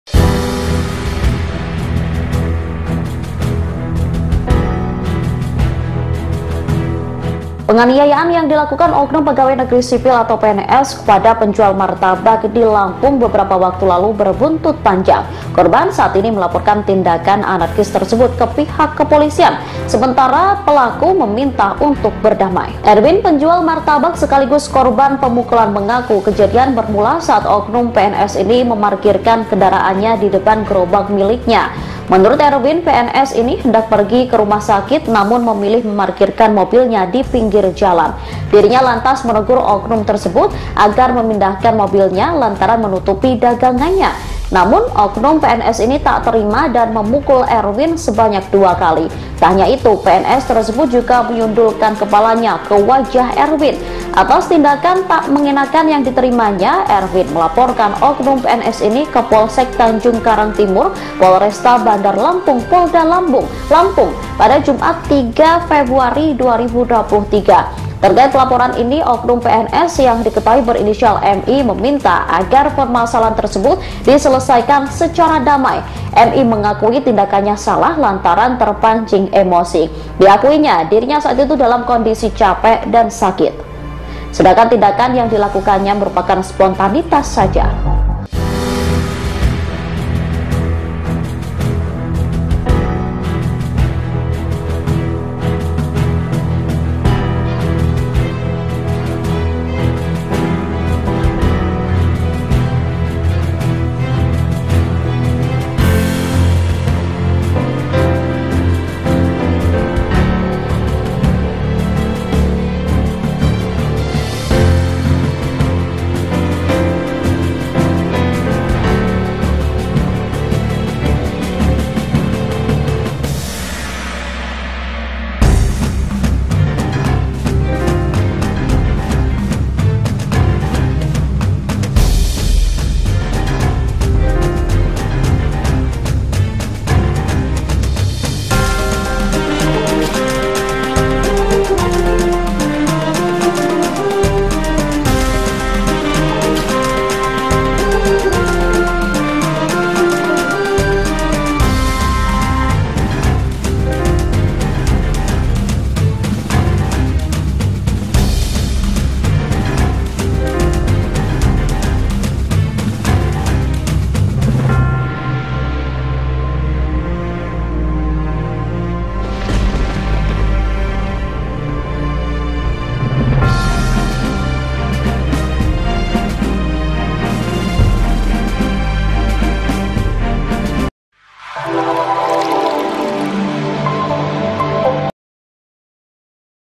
Oknum PNS yang Pukul Pedagang Martabak Dilaporkan ke Polisi, Kini Minta Maaf dan Ingin Damai
Kasus oknum Pegawai Negeri Sipil (PNS) yang melakukan penganiayaan kepada penjual martabak beberapa waktu lalu berbuntut panjang.

Terkait hal tersebut, kini korban melaporkan tindakan anarkis tersebut ke pihak kepolisian.

Penjual Martabak bernama Erwin sekaligus korban pemukulan mengaku, kejadian bermula saat oknum PNS ini memarkirkan kendaraannya di depan gerobak miliknya.